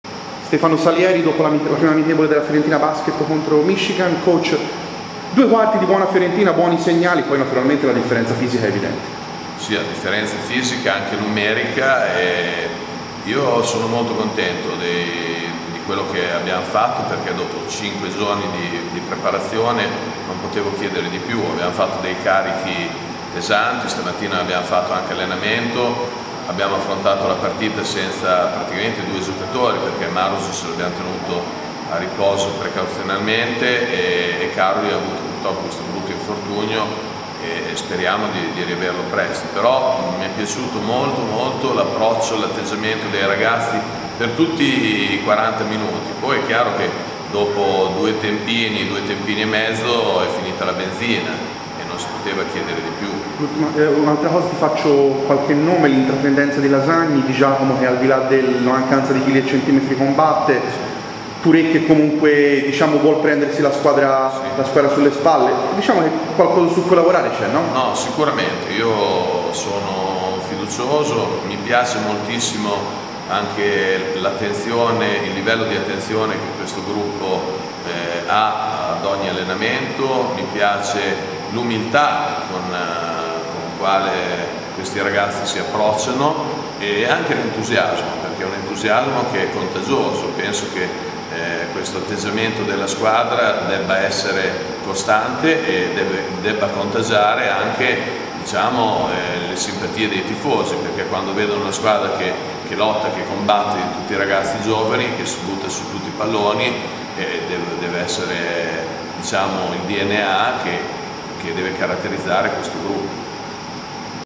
Coach Salieri dopo Fiorentina vs Michigan State
Debutto per la neonata compagine gigliata al PalaCoverciano. Sconfitta contro una tra le principali università degli Usa, guidata da coach Izzo.